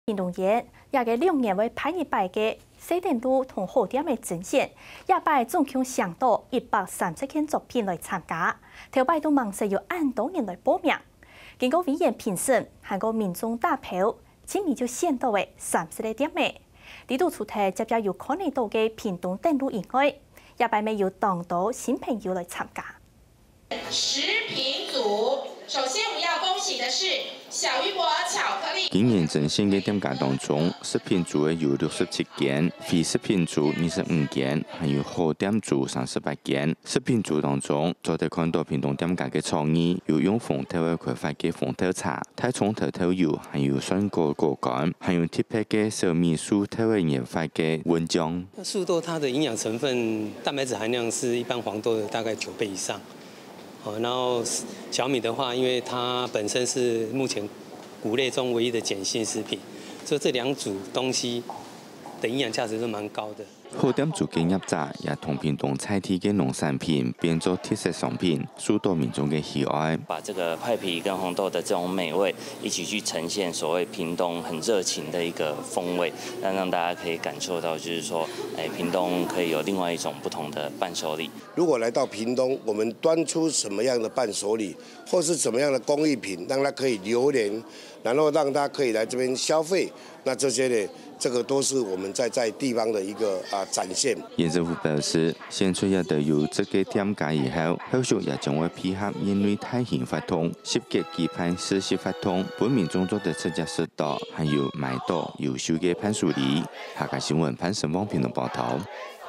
屏東伴手禮暨好店徵選 30名店家脫穎而出【客家新聞20190916】
屏東縣2年一次的伴手禮暨好店徵選，共徵集130件參賽作品，件數創歷年新高，歷經委員評審及民眾投票，最後共30名店家脫穎而出，除了屏東好禮的常客之外，也多了不少新面孔。